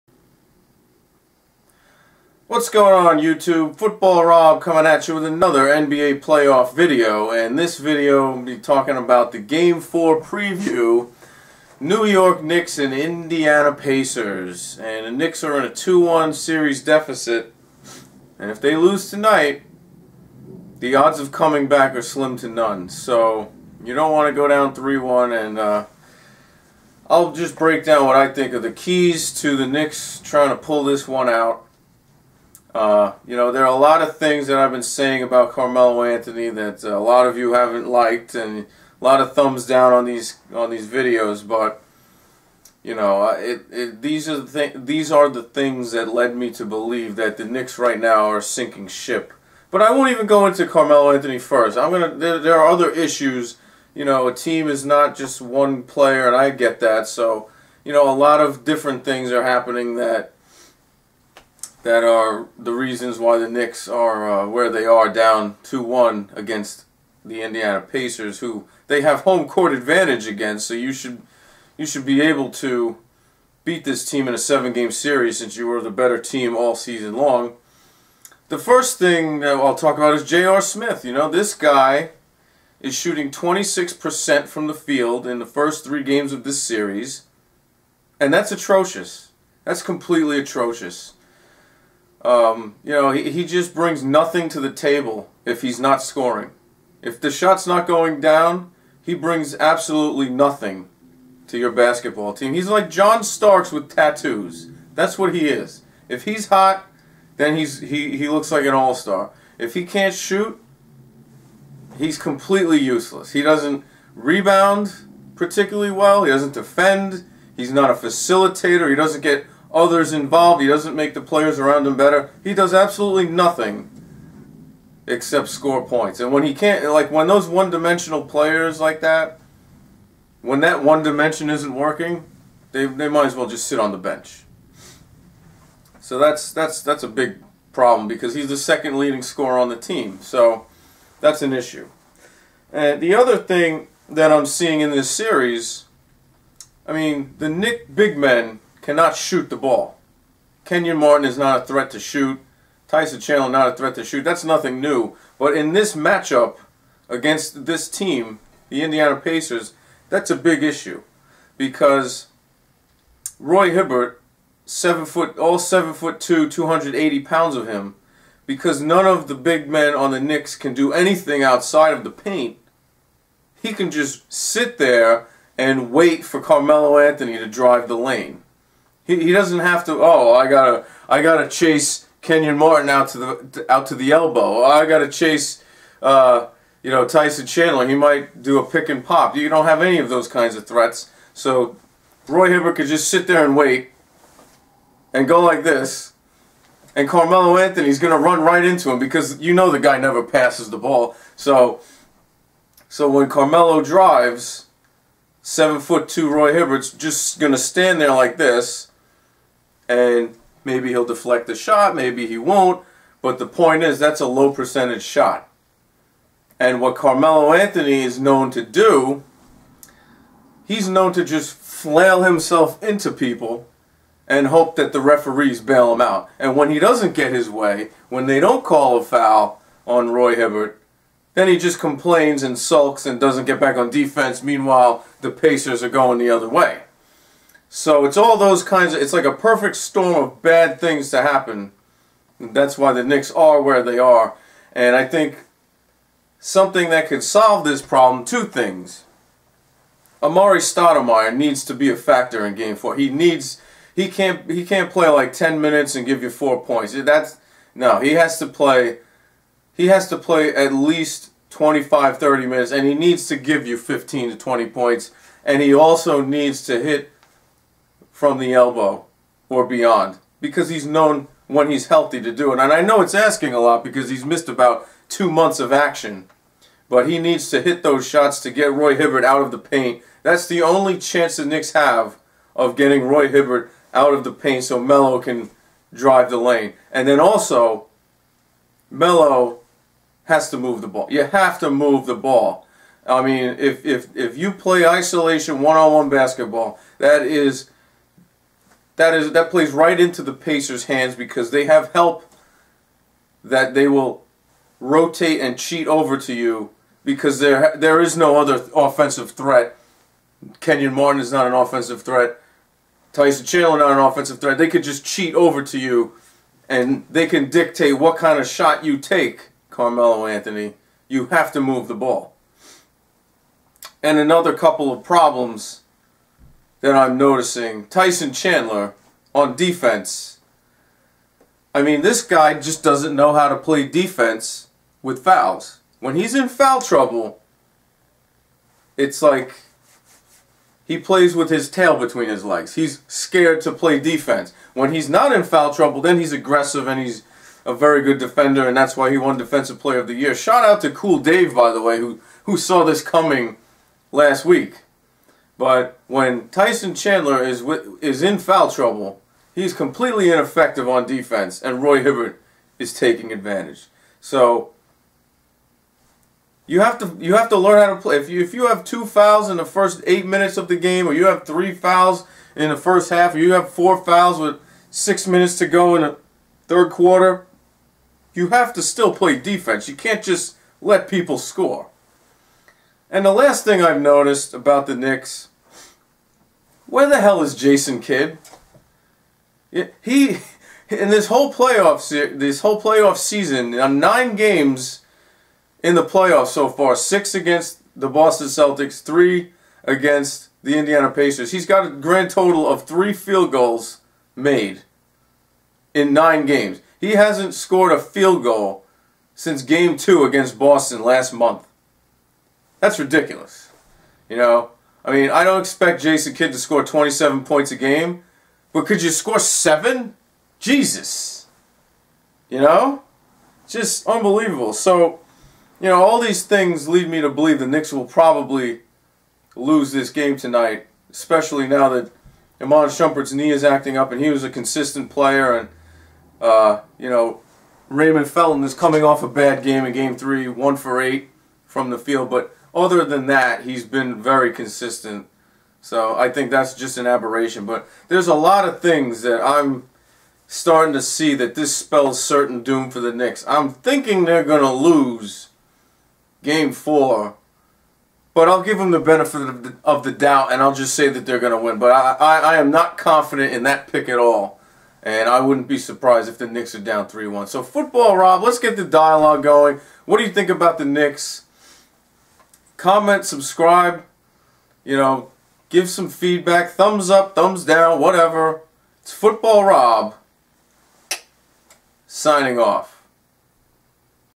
New York Knicks: 2013 Game 4 Preview
I break down the keys to victory for the New York Knicks against the Indiana Pacers in Game 4 of the Eastern Conference Semi-Finals.  The Indiana Pacers hold a 2-1 series lead at the time of this video.